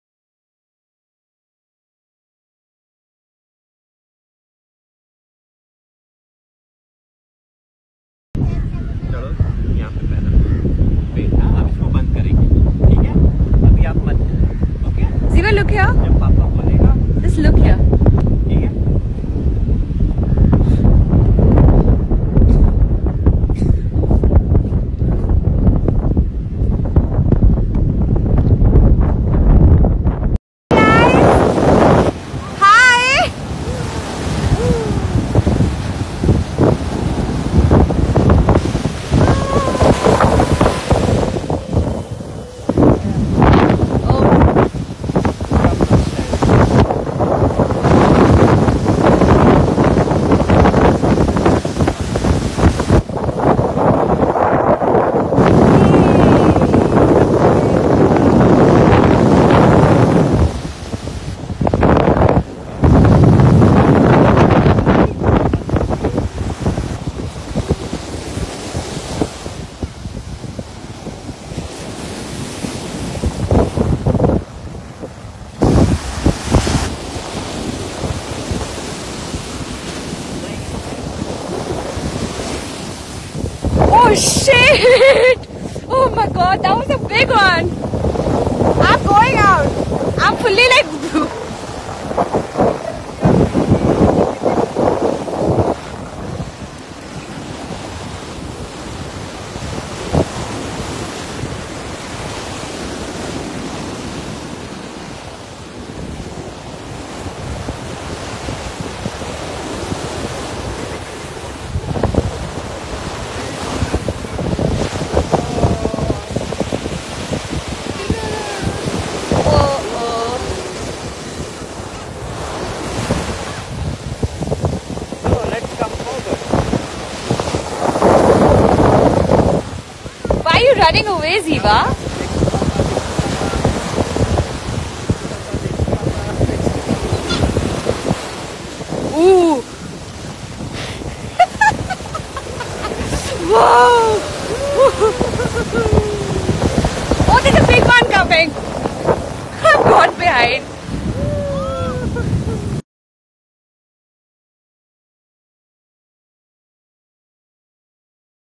Dhoni and Daughter Chennai Marina Beach Playing in Enjoy Moment Viral Video
Dhoni and Daughter Chennai Marina Beach Playing in Enjoy Moment Troll
dhoni,shakshi dhoni and Ziva chennai books opening ceremeny.